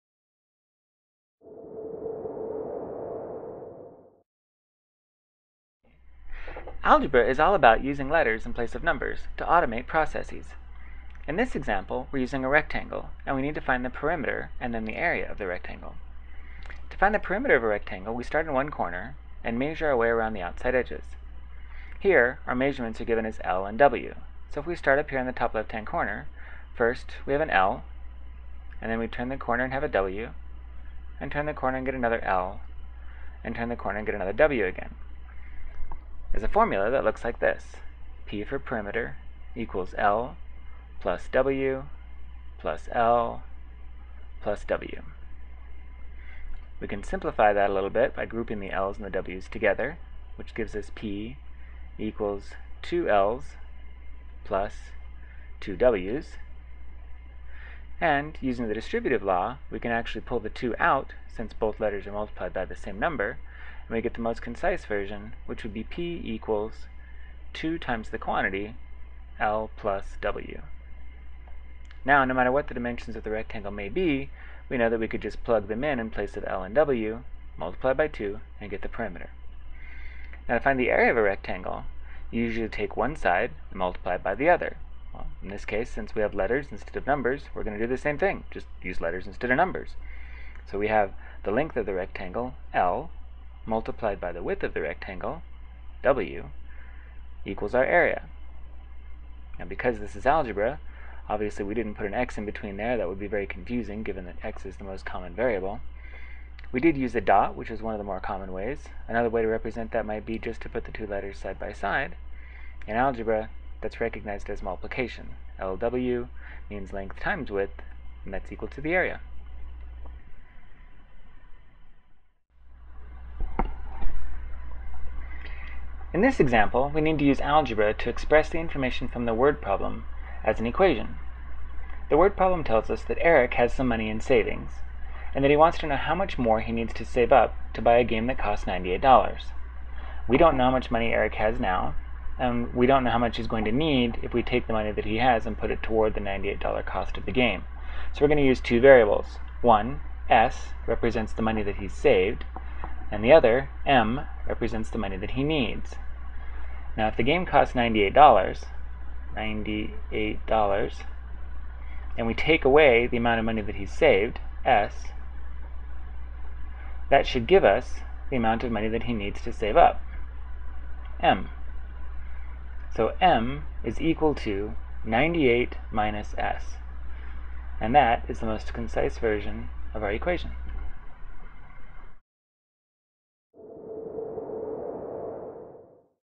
The Language of Algebra: An Explanation (Algebra I)
Here you'll learn how to represent unknown quantities with variables so you can write algebraic expressions and equations for real-world situations.

This video provides an explanation of the concept of the language of algebra.

This is part of CK-12's Algebra I: Equations and Functions.